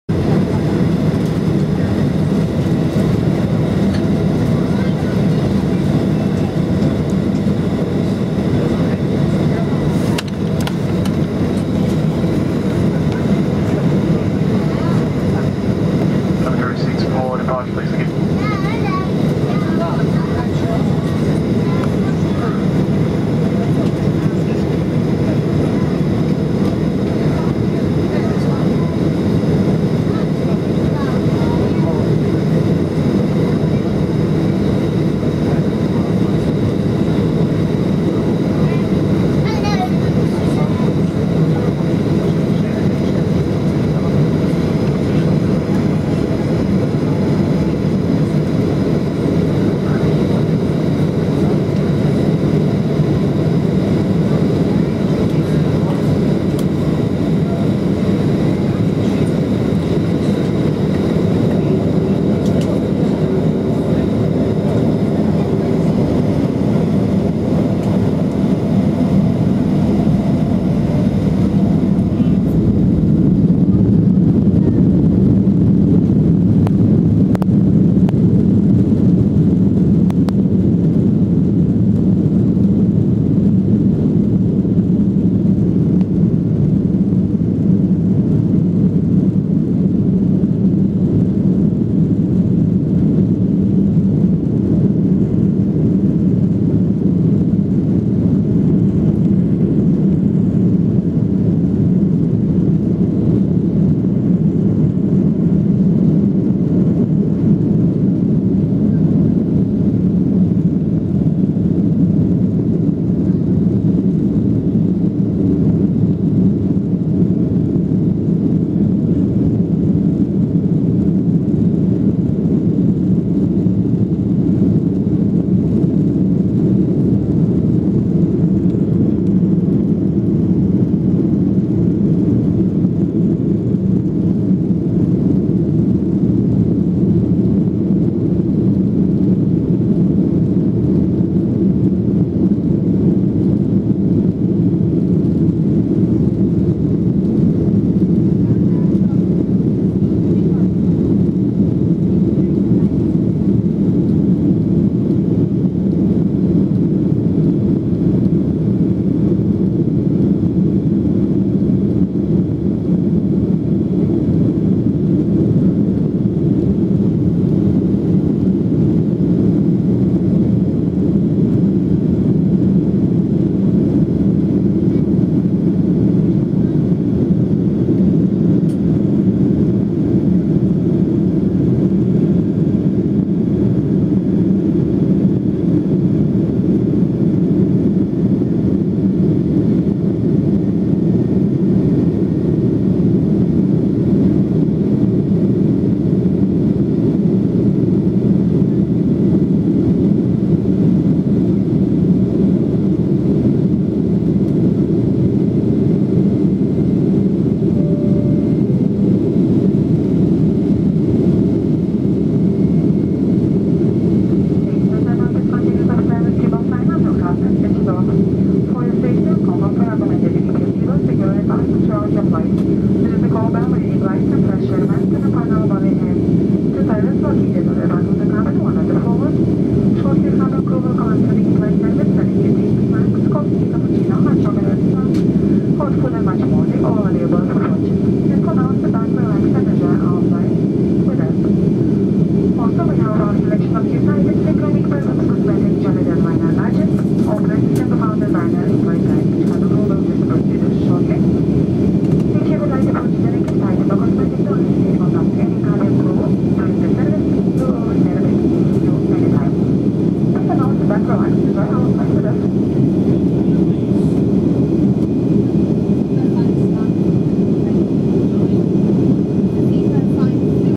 Night Departure Ryanair Boeing 737 at Manchester Airport
On-board Ryanair FR559 Taxing and Taking off at Manchester Airport, England on Thursday 16th July 2015.

It was a good flight on Ryanair our aircraft, EI-DAO. We were 15 mins minutes departing to our schedule time was 22:10pm.  A fairly smooth 35 minute journey. Our Arrival was smooth and we decented just by Liverpool Airport.

This Ryanair Aircraft, EI-DAO was delivered on the 05. Sep 2003 and she is powered by 2 CFM56-7 series engines.

Plane: Boeing 737-800
Airline: Ryanair
Reg: EI-DAO
Flight Number: FR552
Seat number: 31A
Route: Manchester-Dublin
Cruise level: 26,000 feet
Date: 16th July 2015
Departure time: 22:10pm (local time)
Arrival time: 23:55pm (local time)
Flight duration: 35 minutes.